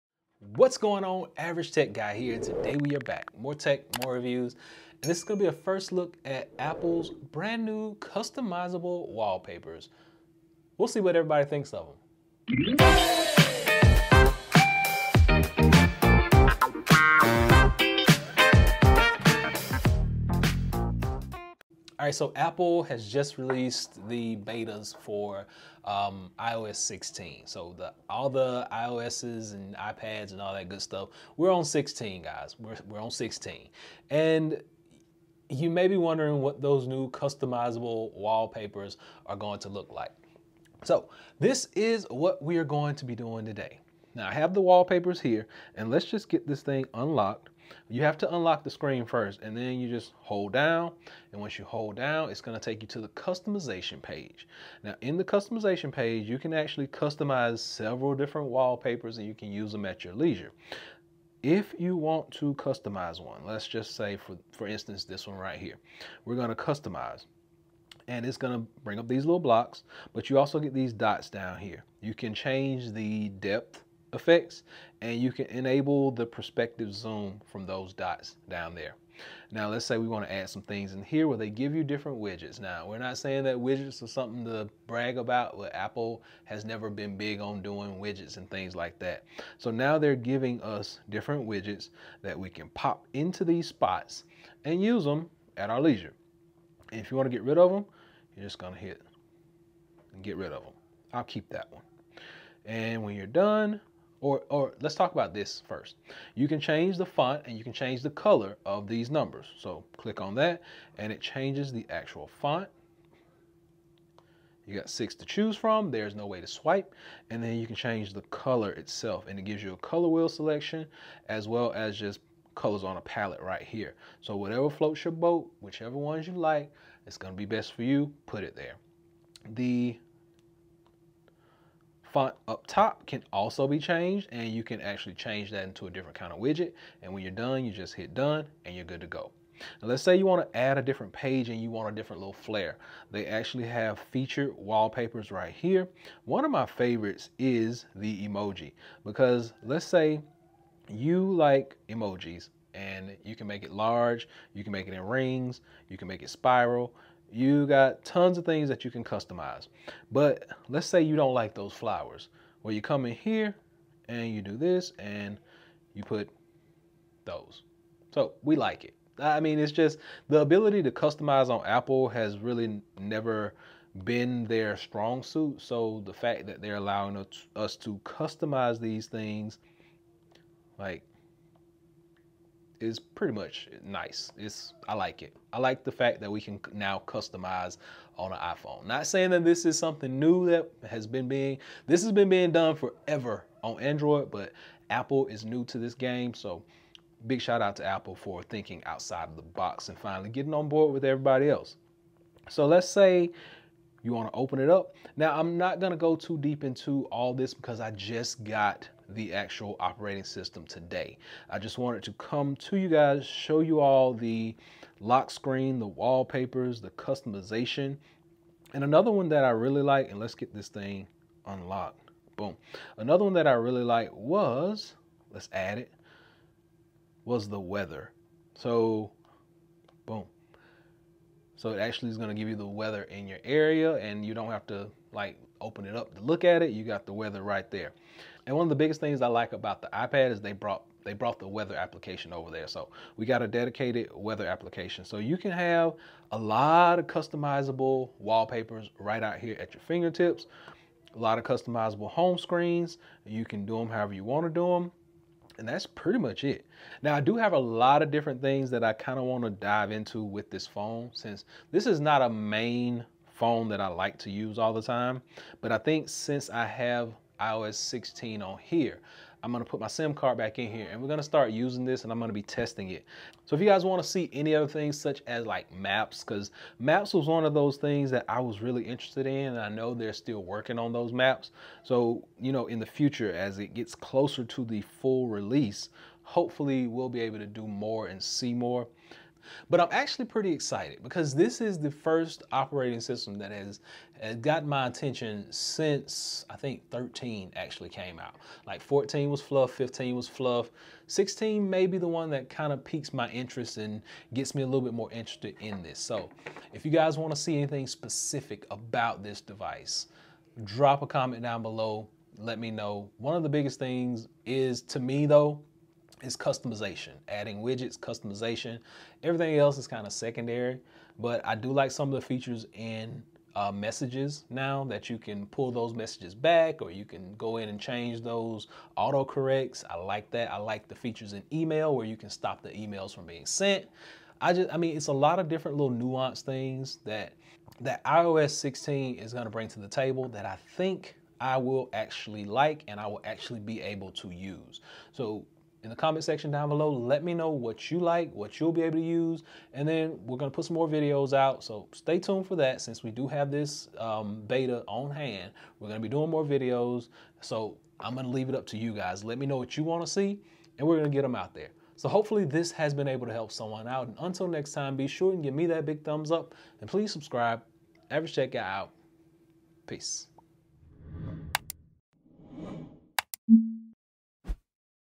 iPhone 13 Pro Max iOS 16 Lockscreen Customization Preview...
iOS 16 is around the corner and here's a first look at it on iPhone 13 Pro Max.

Shop UAG MagSafe Cases Below:
MagSafe Monarch

Zugu Case:
Code ATG2022 for 15%off

Get Your iPad Here:
iPad Pro 11